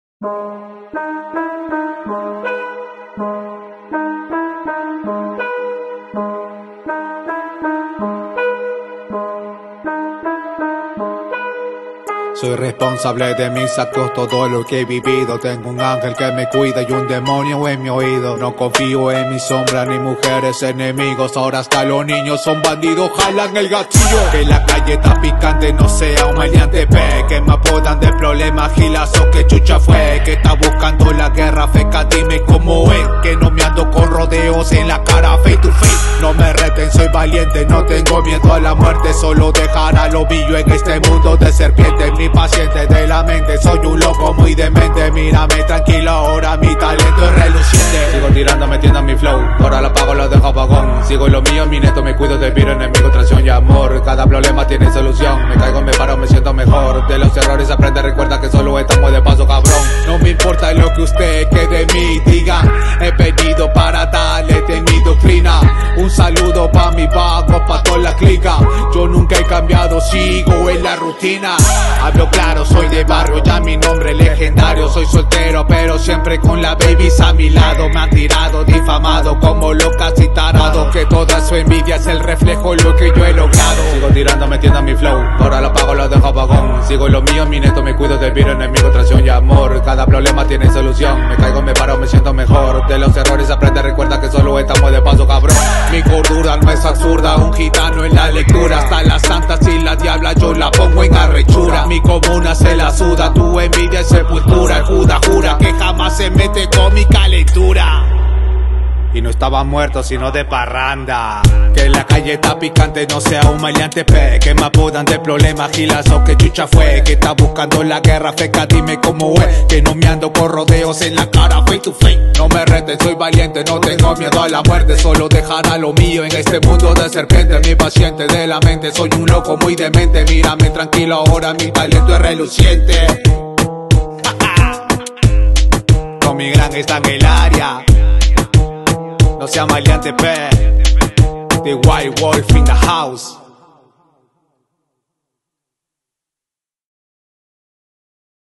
Nikon D7500 Trap Latino Music Video
Hey Guys Another work i made for Tommy EL Grand
Gear - Nikon D7500 - Tokina 11-16mm F2.8 - Sigma Art 30mm f1,4
Flycam C5 for stabilization
Directed By Kanashiro Walter
Thanks Always for watch ;D